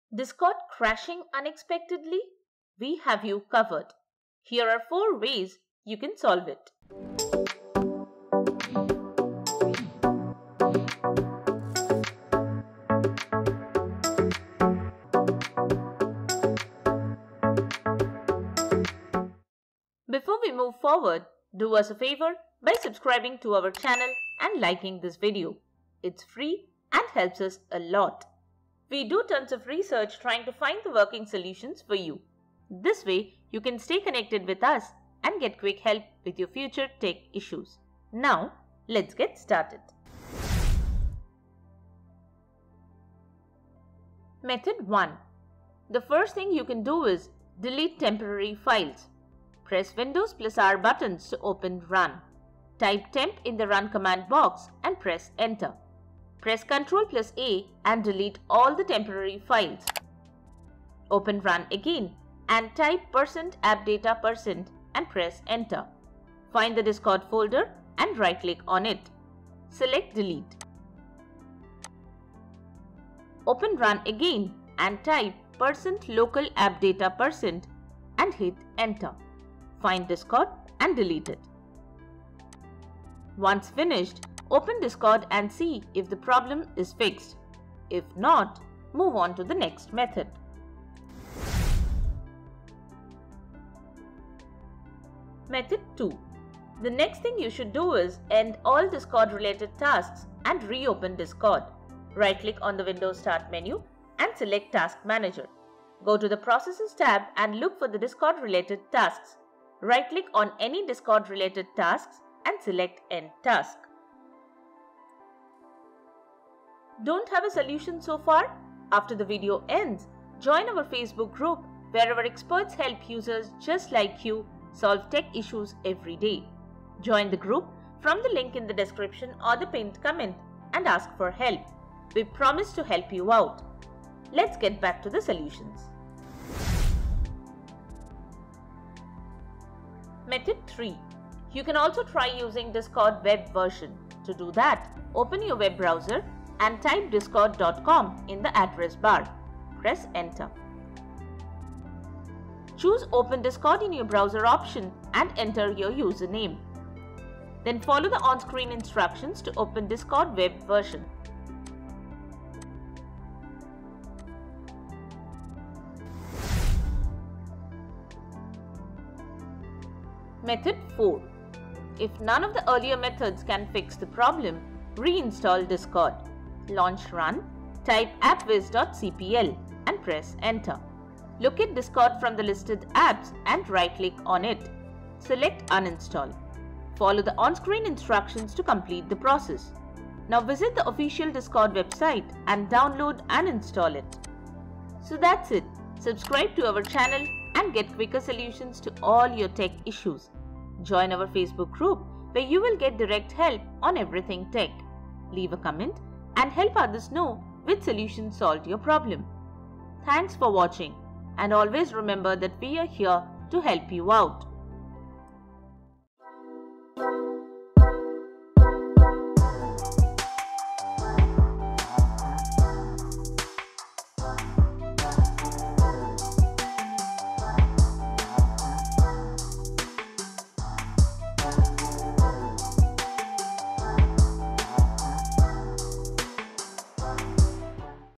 FIX 'Looks Like DISCORD Has CRASHED Unexpectedly' [Working Methods]
Is Discord crashing unexpectedly? We have you covered!
Here are 4 ways you can solve it.

Timestamps:
00:00 Intro
00:42 Delete Temporary Files
01:33 End Discord Related Tasks
02:18 Use Discord Web Version
02:48 Re-install Discord